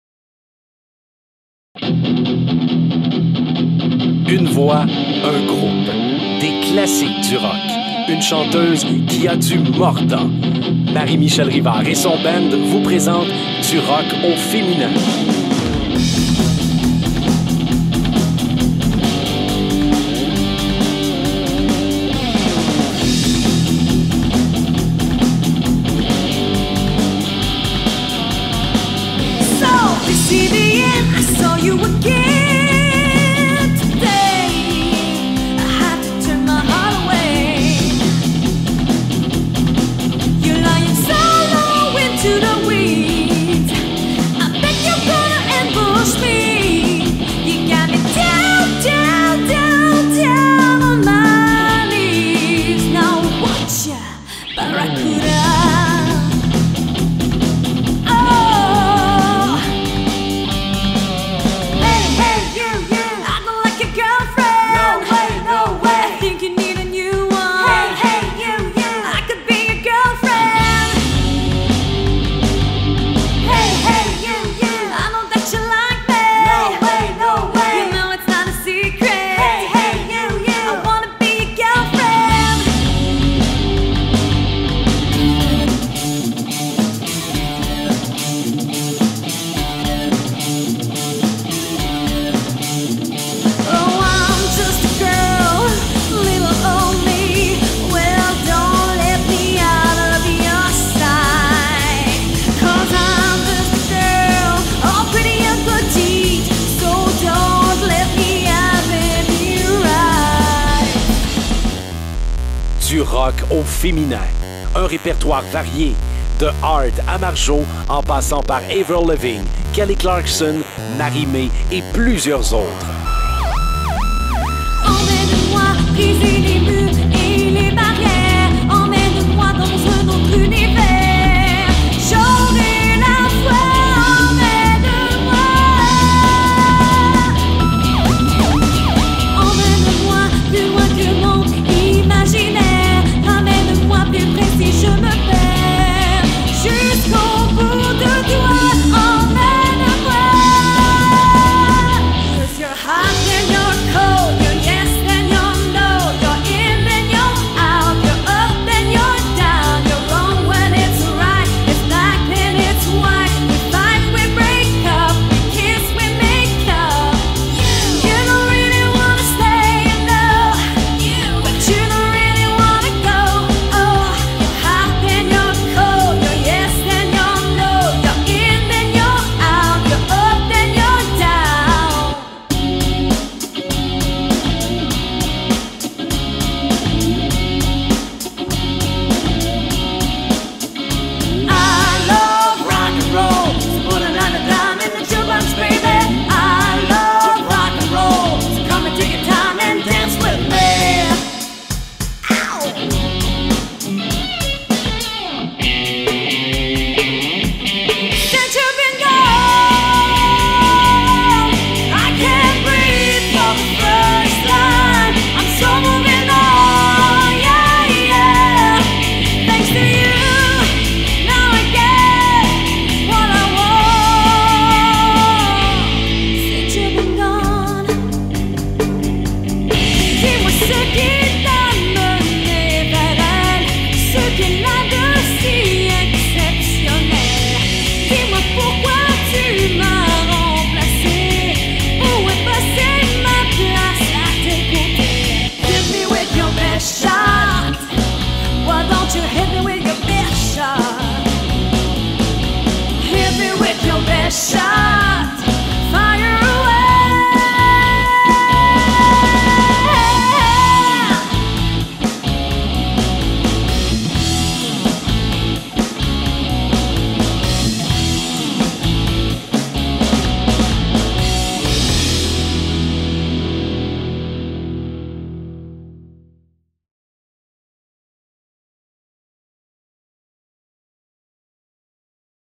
Du ROCK au Féminin Vidéo promotionnel
Groupe rendant hommage aux femmes du rock . Des années 70 jusqu'à aujourd'hui : Heart , Sass Jordan , Janis Joplin , Marjo , Nanette Workman , Avril Lavigne , Kelly Clarkson , Marie-Mai, etc ...